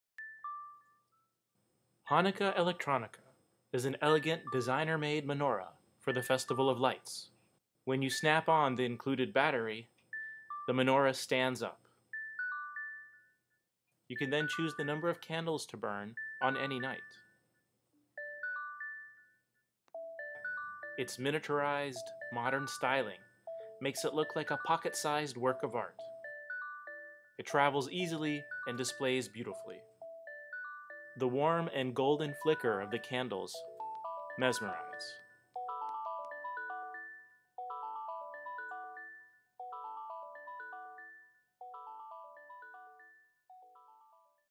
Hanukkah Electronics, Intro Video
Introducing the Hanukkah Electronica, a stylish LED pocket-sized menorah for the festival of lights.